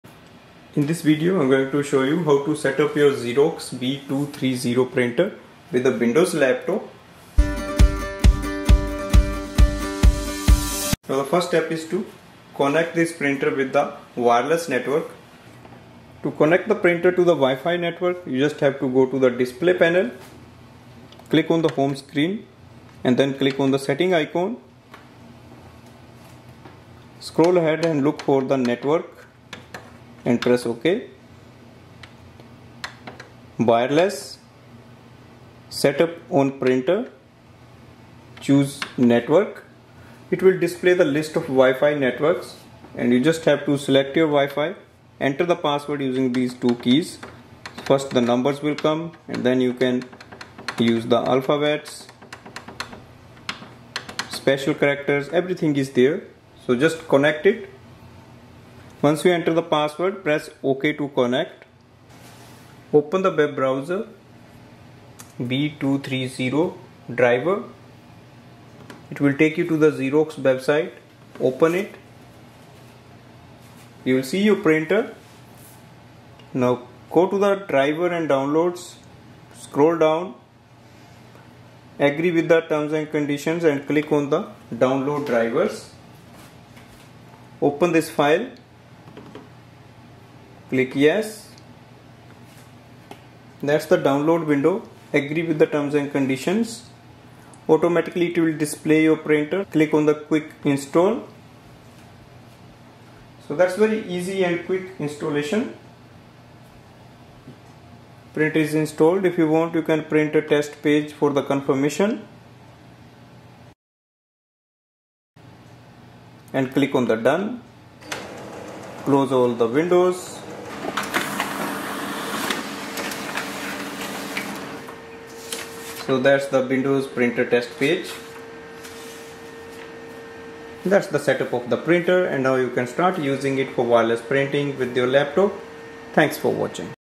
Xerox B230 Setup Win Laptop, Wireless Setup, Install drivers, Print From Laptop !
This video reviews how to set up your Xerox B230 Laser Wireless printer with windows laptop. This shows how to connect the printer to the home or office wireless network, install drivers in laptop and Windows 11, wireless printing test.